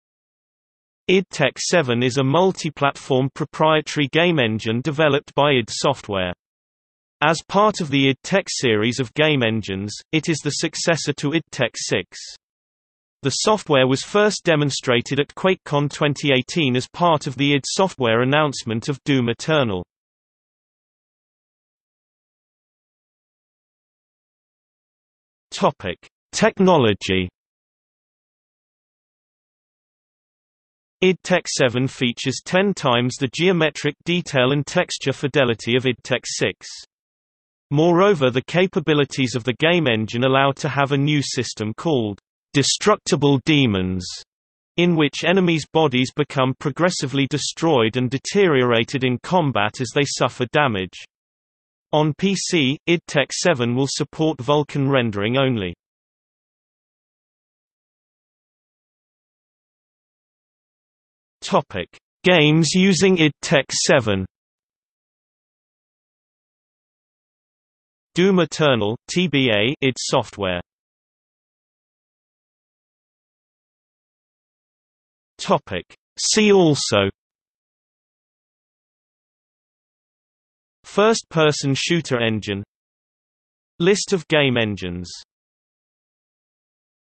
Id Tech 7 | Wikipedia audio article
00:00:21 1 Technology
00:00:57 2 Games using id Tech 7
00:01:11 3 See also

- Socrates

SUMMARY
id Tech 7 is a multiplatform proprietary game engine developed by id Software. As part of the id Tech series of game engines, it is the successor to id Tech 6. The software was first demonstrated at QuakeCon 2018 as part of the id Software announcement of Doom Eternal.